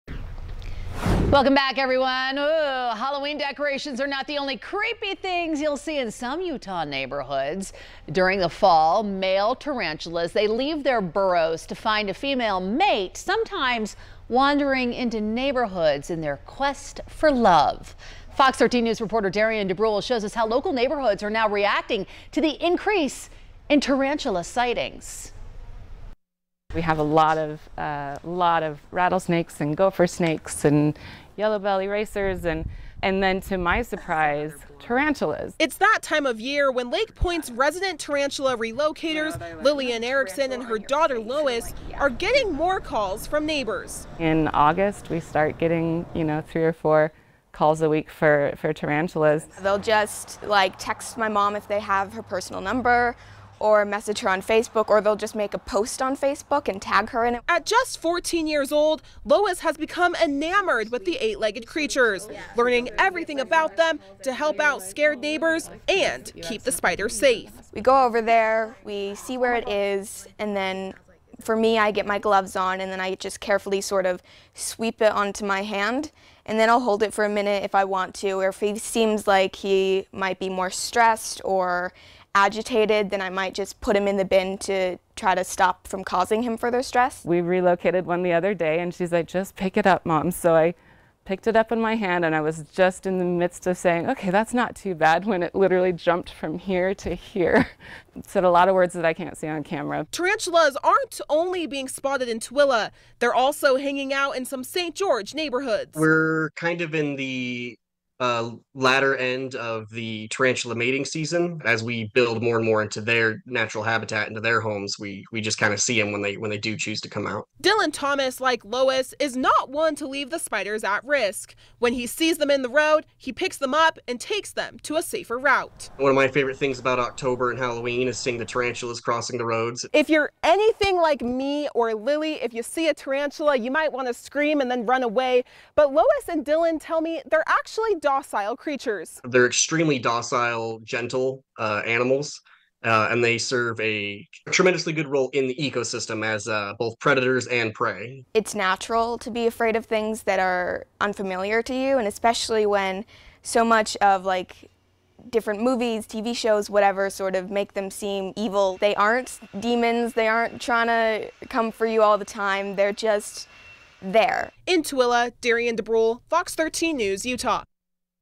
Utah tarantulas looking for love during Halloween, not to scare
During the fall season, male tarantulas leave their burrows to find a female mate, sometimes wandering into Utah neighborhoods in their quest for love.